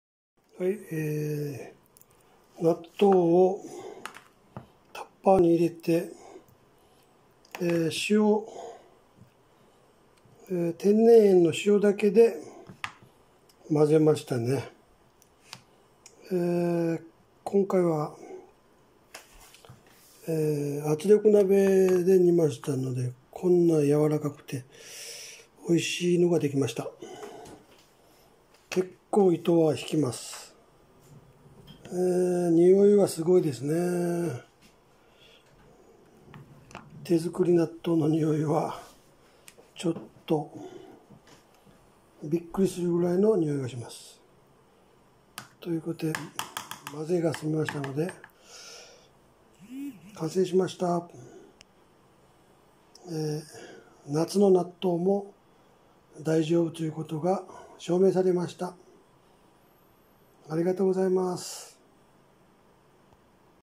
😲納豆が熟成しました😂
😲手作り納豆が😃熟成しましたので😂天然塩だけで味付けします😋